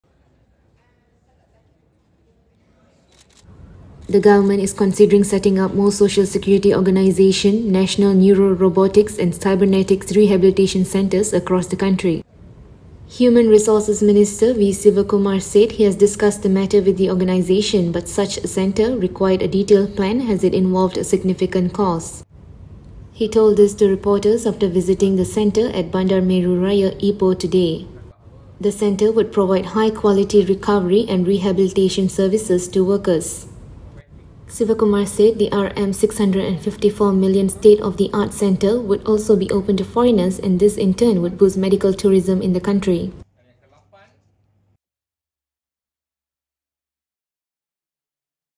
More National Neuro Robotics and Cybernetics Rehab Centres being mulled nationwide
The government is considering setting up more Social Security Organisation (Socso) National Neuro-Robotics and Cybernetics Rehabilitation Centres across the country.